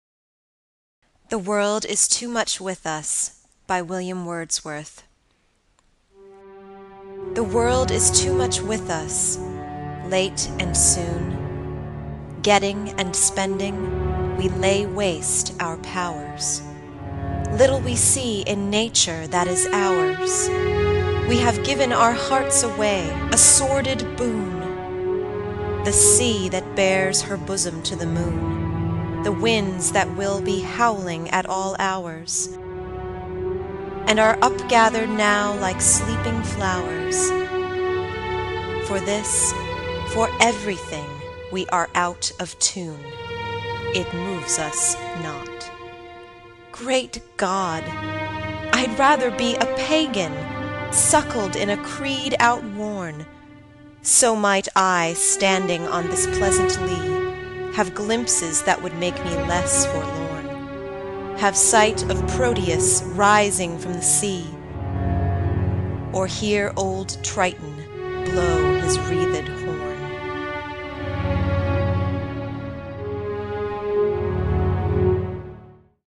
英文诗歌朗读：威廉·华兹华斯《世界于我们太过纷杂》|The World Is Too Much With Us by William Wordsworth
威廉·华兹华斯（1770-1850）是英国著名浪漫主义诗人，是文艺复兴运动以来最重要的英语诗人之一。这首《世界于我们太过纷杂》想表达的意境与陶渊明的《归园田居》有异曲同工之妙，在纷繁的生活中，诗人更想亲近自然。